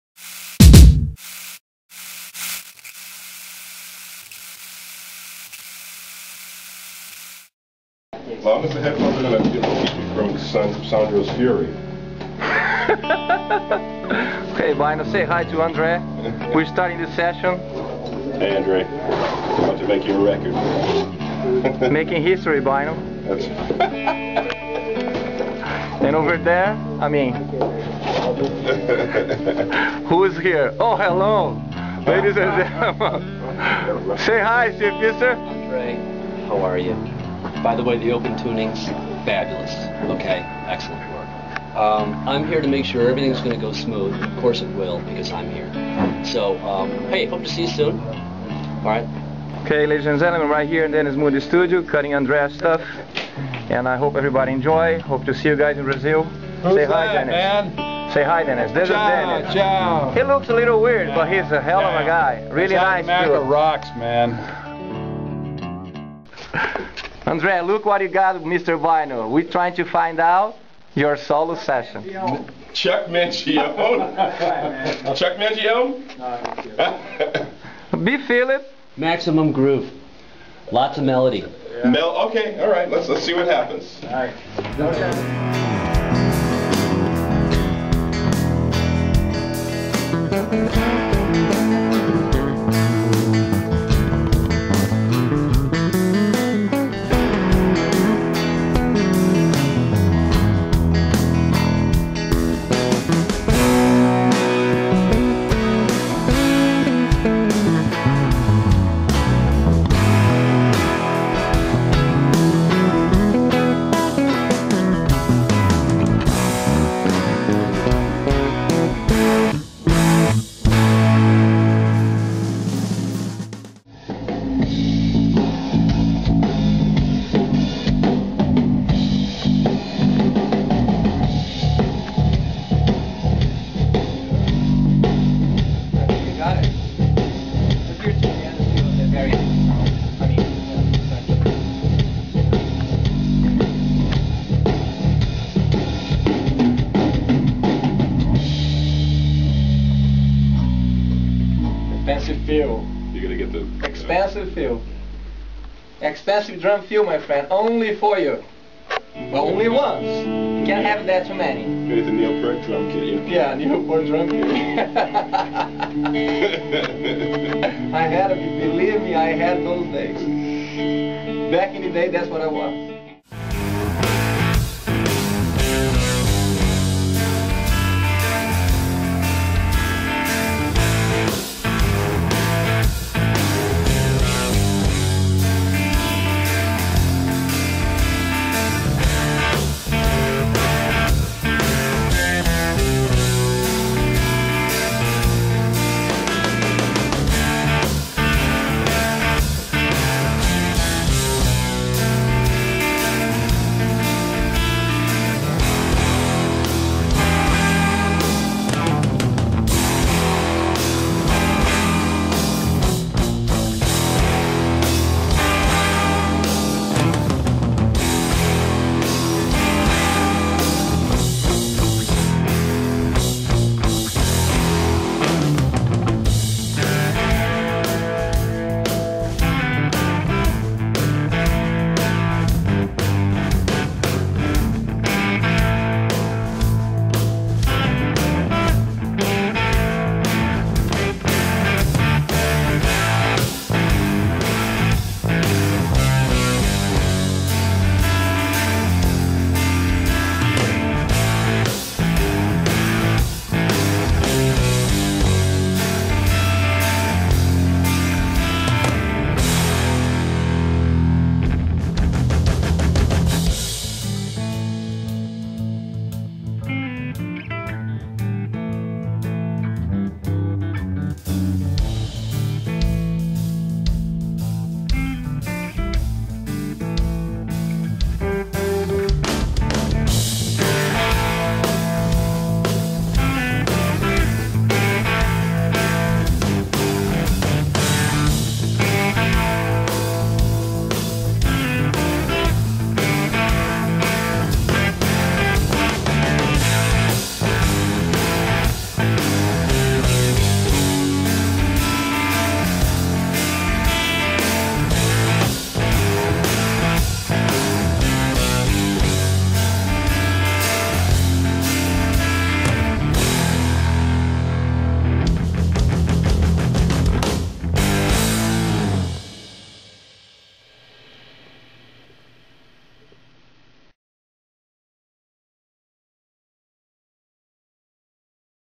Andre Sampaio Recording
Primeiro dia de gravação do meu CD no Dennis Moody Studio (Los Angeles, CA). Sandro Feliciano - Drums Philip Bynoe - Bass Participação especial - Steve Fister Para mais informações acesse